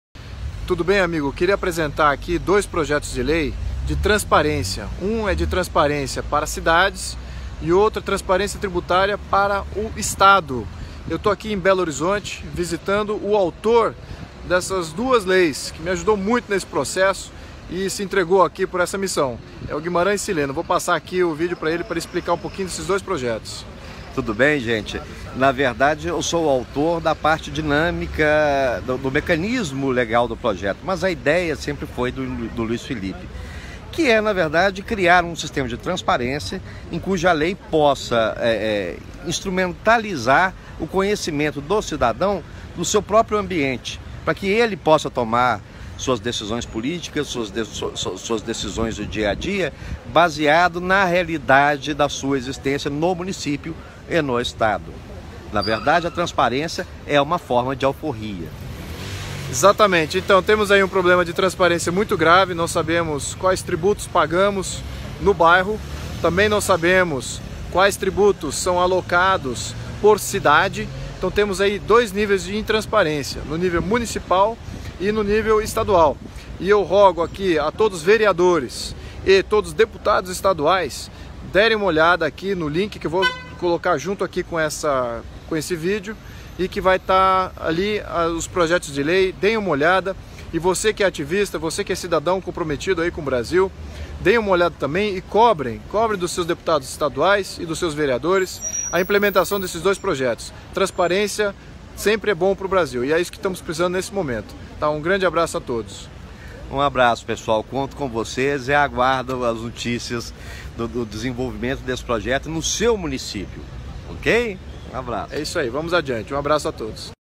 Lançamento — O Brasil precisa de transparência tributária
Lançamento: O Brasil precisa de transparência tributária, e isso vai muito além de informar o valor dos impostos no cupom fiscal. É antidemocrático. Sem isso o povo não pode ajudar ao poder público a fazer as melhores decisões. A cobrança fica vaga, não estimula o dialogo e a população fica separada do seu próprio dinheiro. Temos uma lei para transparência de tributos do município e uma lei para transparência de tributos no nível estadual. A mudança que queremos requer transparência. Assista o vídeo acesse o link do site para avaliar os projetos. Compartilhe com seu vereador e seu deputado estadual.

Frente Monárquica Brazileira
   ✯ "In hoc signo vinces" ✯